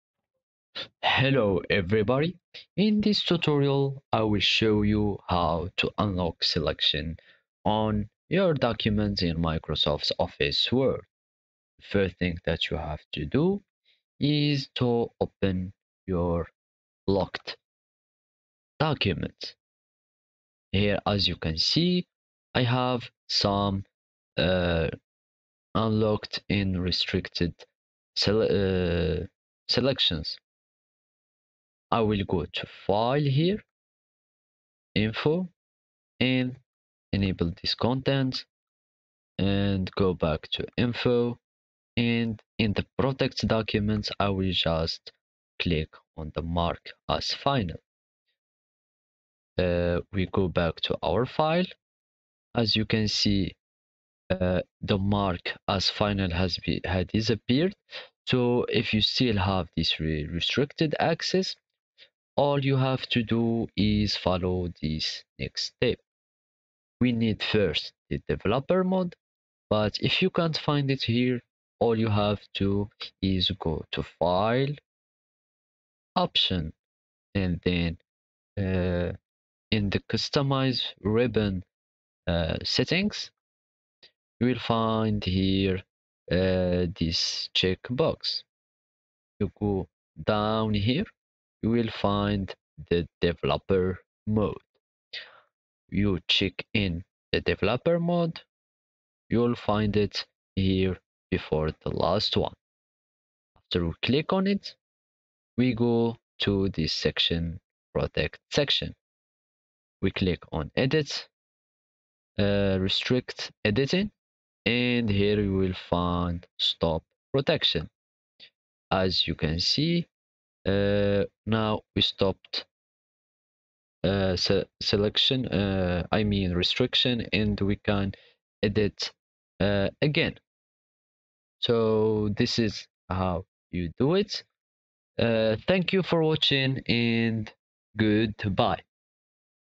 How To Unlock Selection Microsoft Word Tutorial
Today we talk about how to unlock selection in microsoft word,selection is locked microsoft word,microsoft word selection is locked how to unlock,unlock selection microsoft word